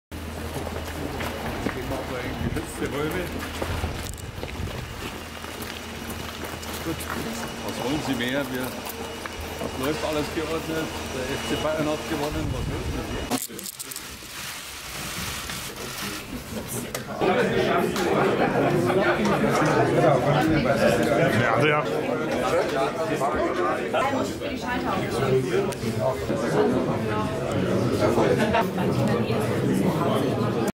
Horst Seehofer - ein schwerer Gang in den Landtag
Horst Seehofer - Letzte Stunden eines Ministerpräsidenten

ein schwerer Gang in den Landtag was über die Frage von Horst Seehofers Zukunft als Ministerpräsident entscheidend ist.